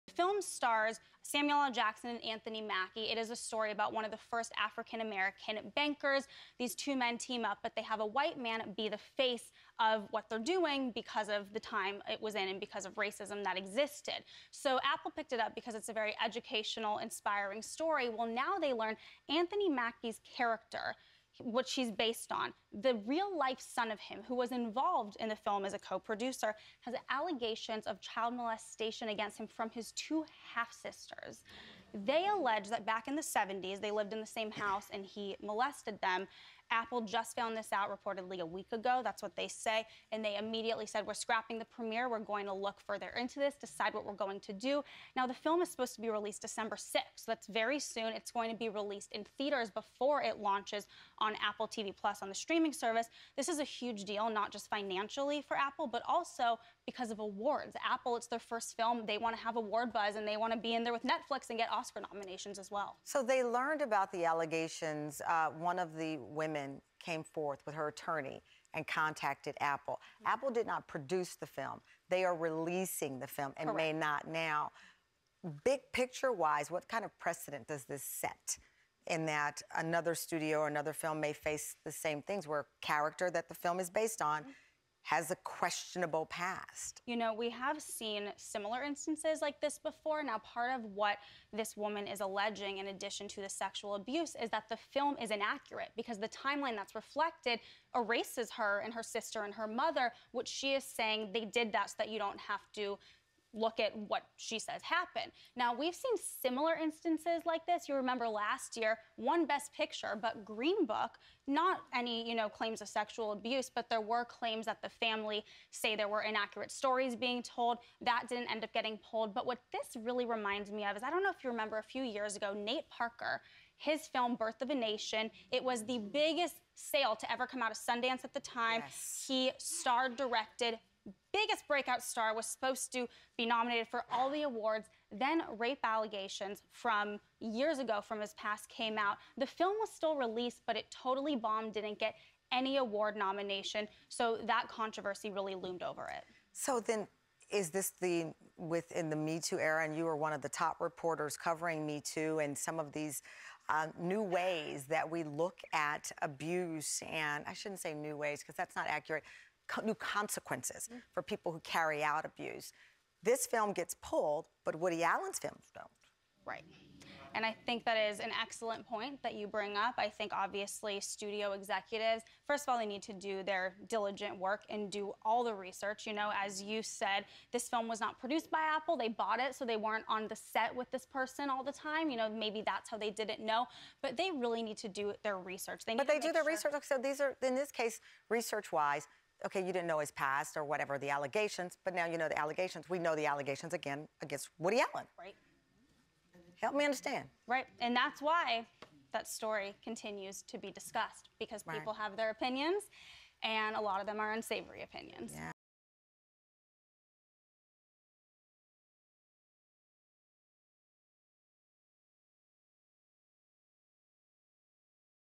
Apple Abruptly Cancels Premiere of Samuel L. Jackson Movie “The Banker”
Variety’s senior correspondent Elizabeth Wagmeister breaks down the details surrounding Apple’s decision to pull new movie “The Banker.”